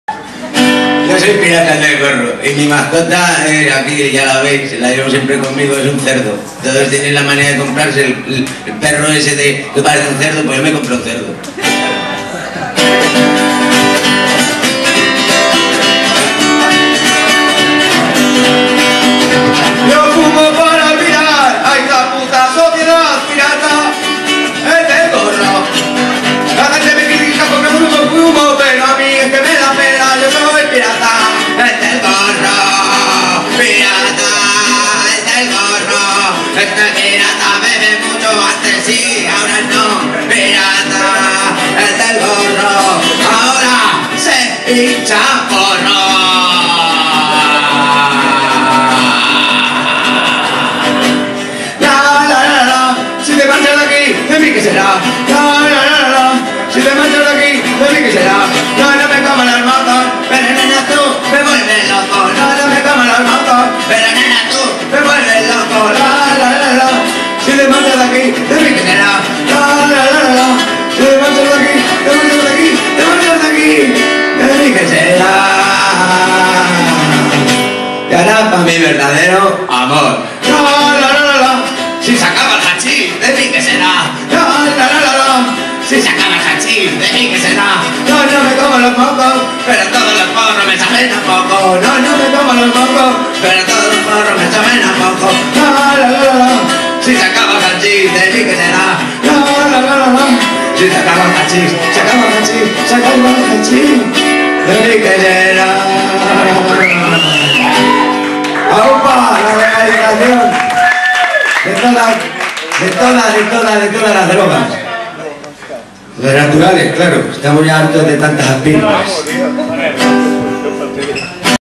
pedg y si te martxas daki -pirata el del gorro- vilanova i la geltru
en esta ocasion en vila-nova i la geltru el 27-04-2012, se tenia de haber hecho en la sala LES GOLFES, y al final por problemas del lokal, se realizo en la discoteca -ARCS